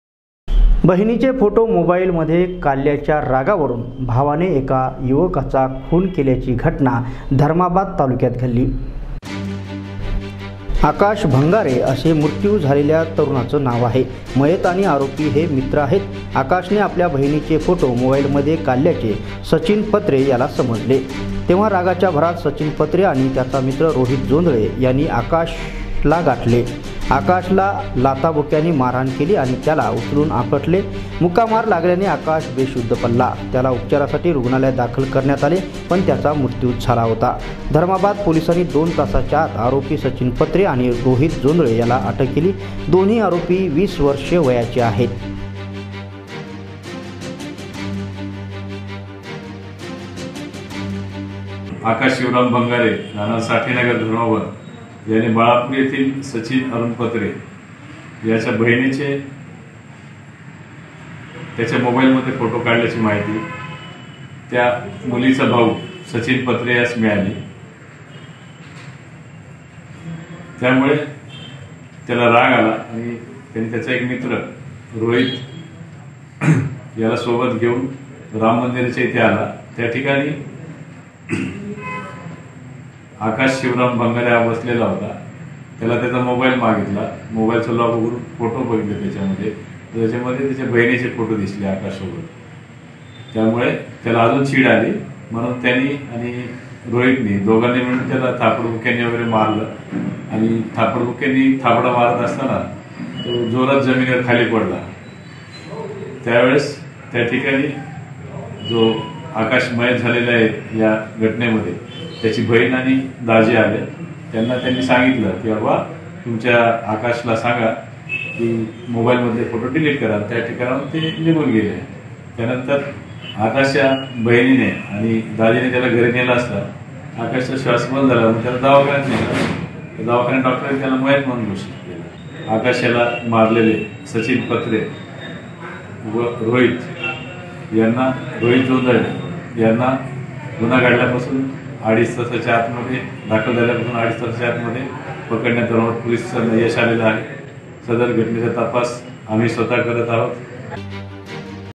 मोबाईलमध्ये बहिणीचे फोटो काढल्याने भावाने केला युवकाचा खुण
भावाने आणि त्याच्या मित्राने तरुणाला केलेल्या मारहाणीत युवकाचा मृत्यू